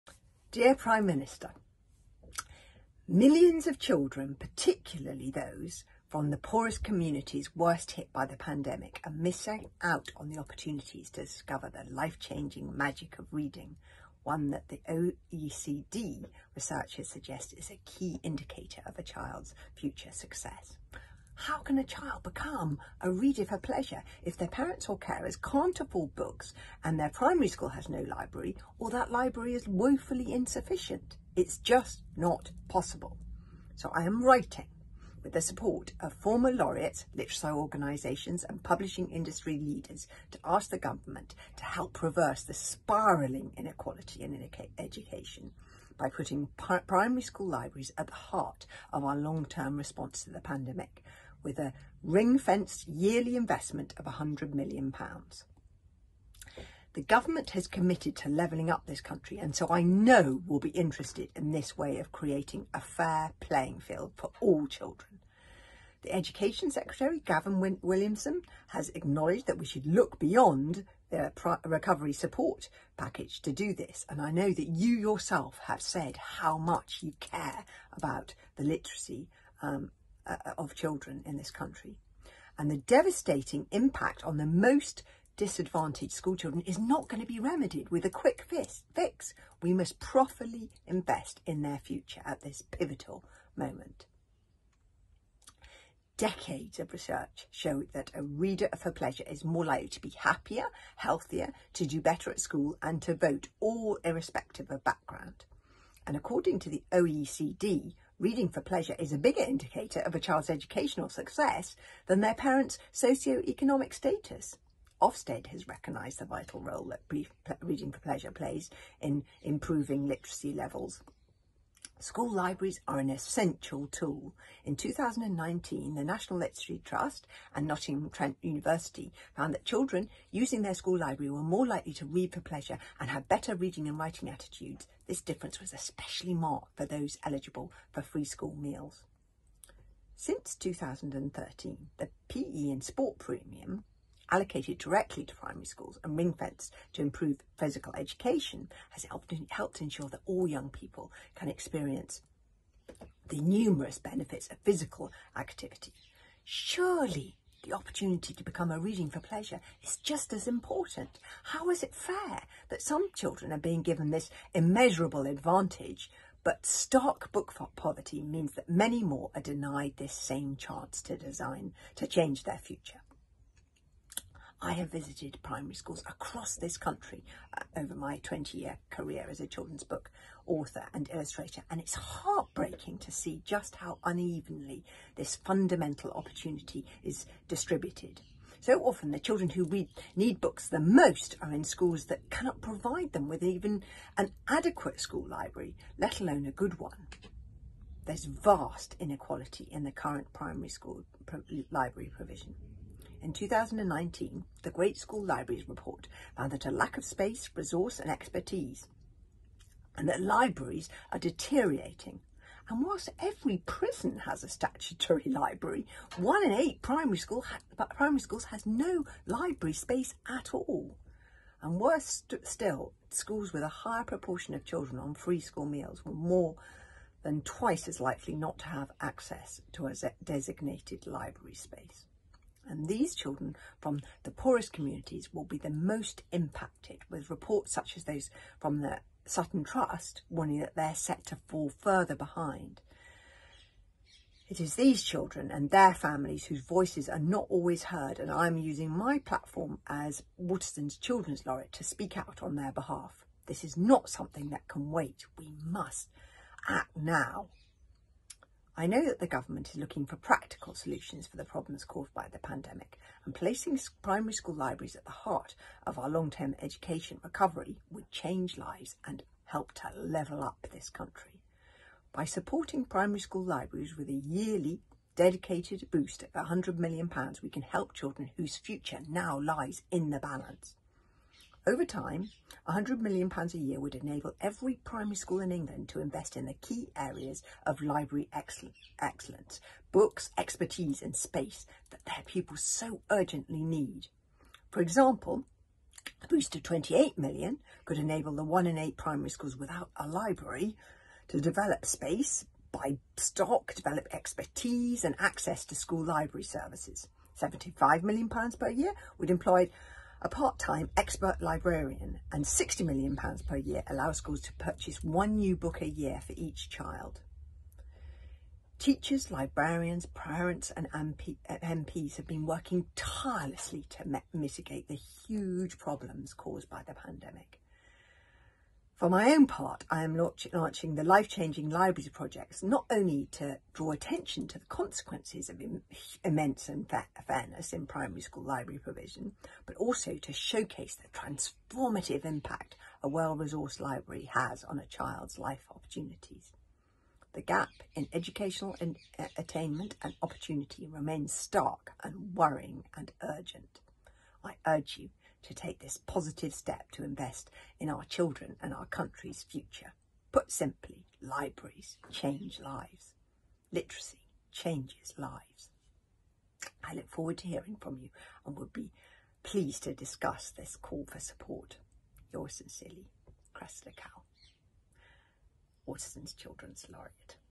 'Libraries change lives': Cressida Cowell's open letter to Prime Minister Boris Johnson
As Waterstones Children's Laureate, Cressida Cowell is launching Life-changing Libraries - a project to highlight the importance of primary school libraries.
Here, she shares her open letter to Prime Minister Boris Johnson, calling for £100m of ring-fenced funding for school libraries every year.